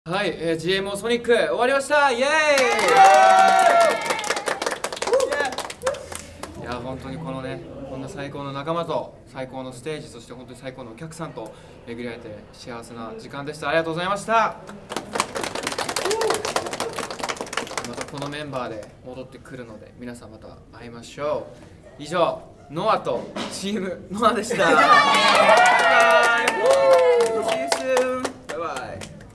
NOAからGMO SONIC 2023の感想動画が到着したよ！
是非チェックしてね👀

sonic.gmo

GMO SONIC 2023
開催日：2023/01/28(SAT) - 2023/01/29(SUN)
会場：さいたまスーパーアリーナ